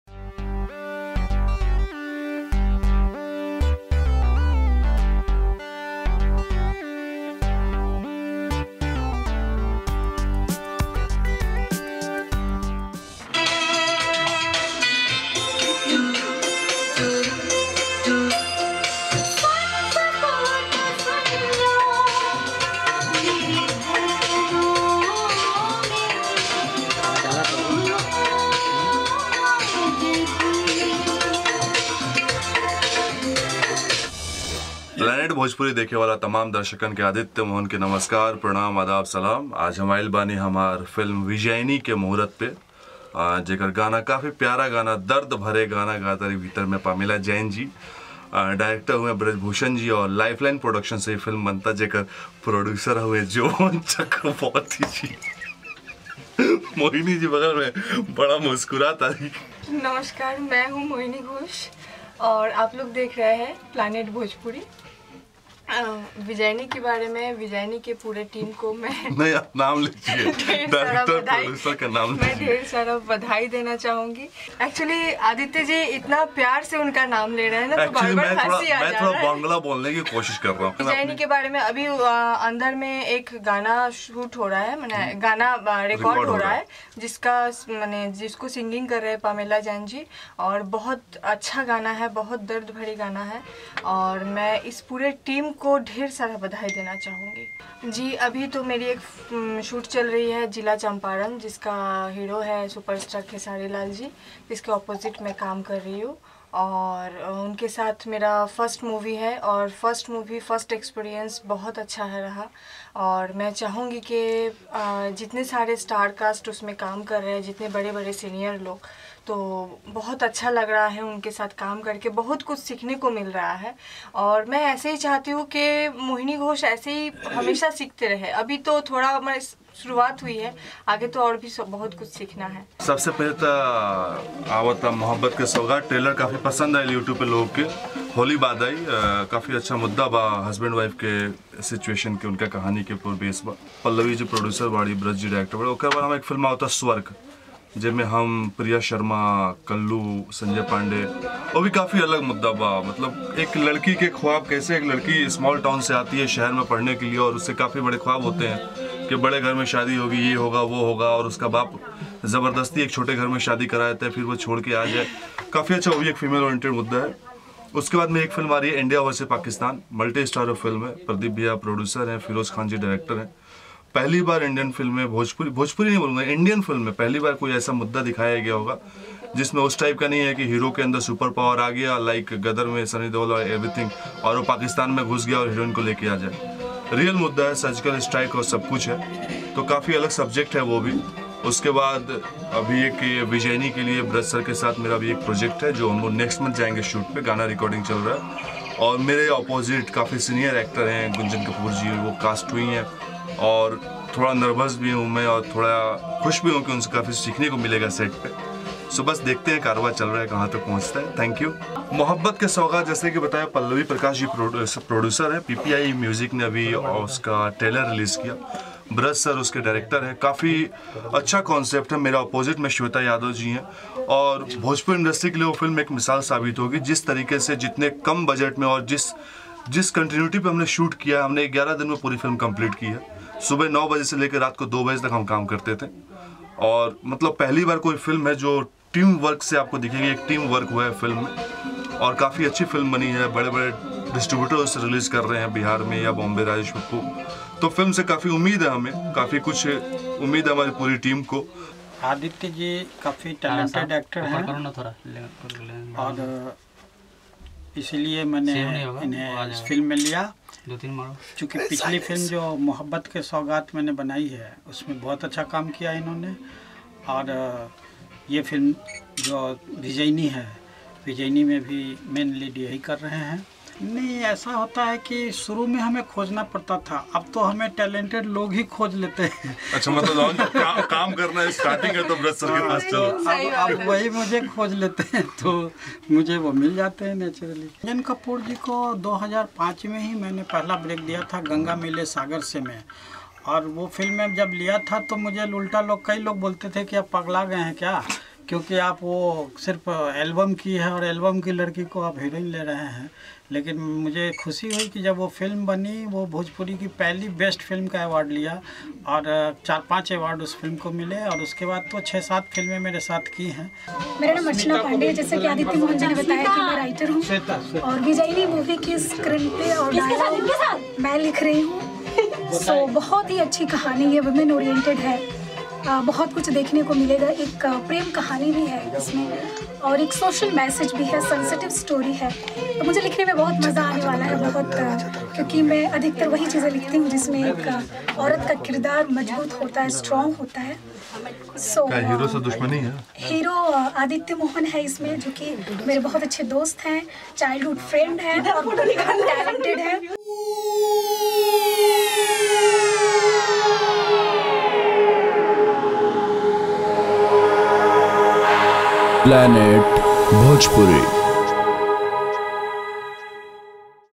विजयनी - Superhit Bhojpuri Film Vijayani - Aditya Mohan Exclusive Interview Muhurat 2017
Planet Bhojpuri is a destination for Bhojpuri Infotainment.विजयनी - Superhit Bhojpuri Film Vijayani -  Aditya Mohan Exclusive Interview Muhurat 2017 on Planet Bhojpuri